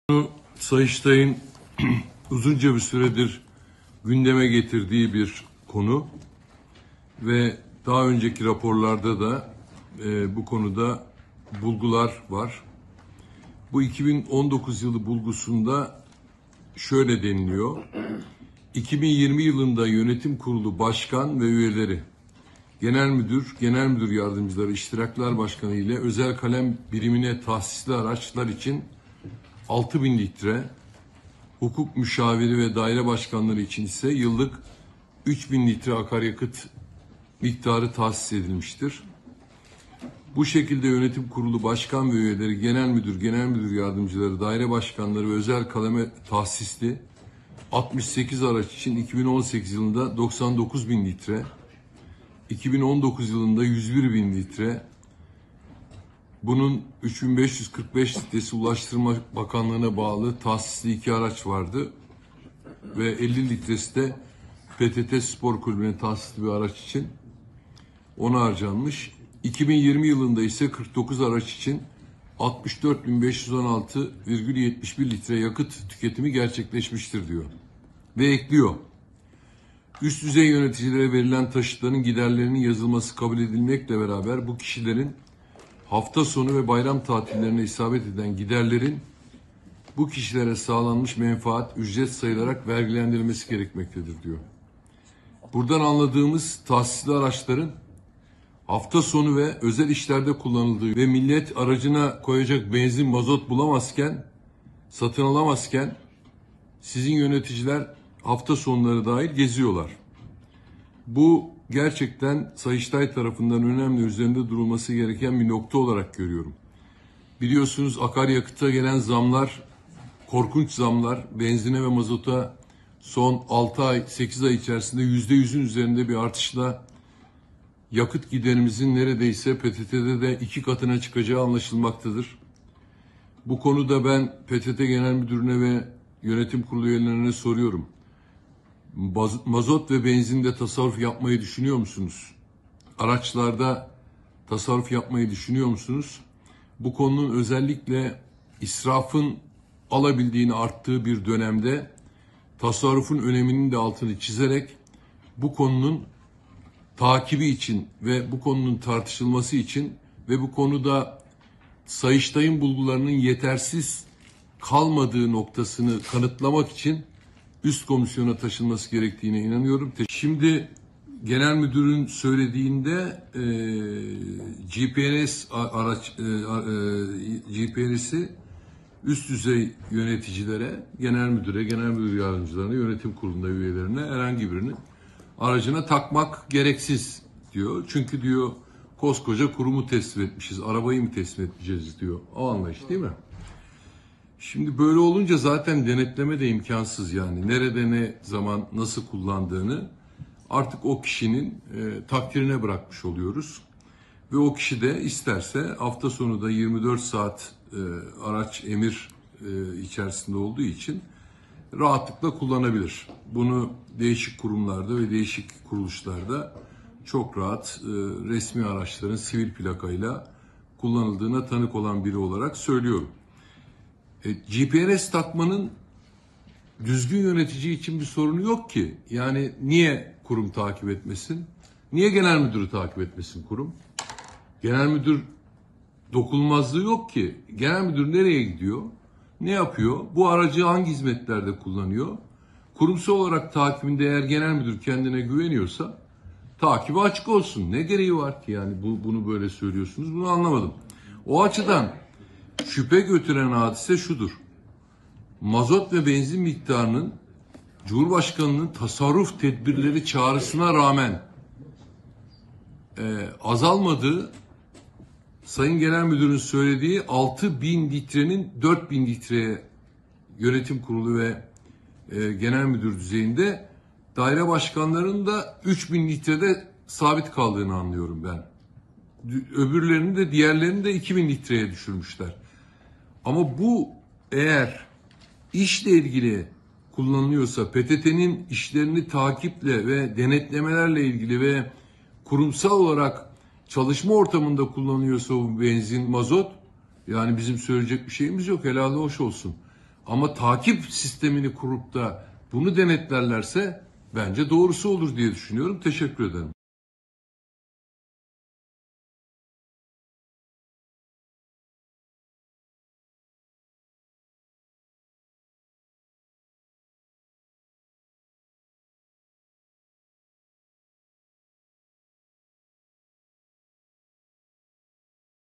CHP'li Atila Sertel, PTT'deki büyük vurgunu ifşa etti: "Sizin yöneticiler..."
CHP'li Atilla Sertel, PTT'deki büyük vurgunu ifşa etti: "Sizin yöneticiler..."

Akaryakıt zamları sonrası PTT’nin KİT Alt Komisyon toplantısında makam araçları için kullanılan yakıt miktarı tartışıldı. CHP İzmir Milletvekili Atila Sertel, Sayıştay'ın makam araçlarının hafta sonu ve bayram tatillerinde de kullanılıyor olabileceğine, bunun yönetici maaşlarından vergilendirilmesine işaret ettiğini belirterek, "Araçlarda takip sistemi zorunlu olmalı ve yakıt israfının önüne geçilmeli. Benzin ve mazotun litresinin 16 lirayı geçtiği şu günlerde kimsenin devletin aracını özel aracı gibi kullanmaya hakkı yok. KİT Komisyonu toplantısında PTT Genel Müdürü Hakan Gülten’e de bu öneriyi getirdim. 'Üst düzey yöneticilerin araçlarının takip edilmesine gerek duymadıklarını söyledi" dedi.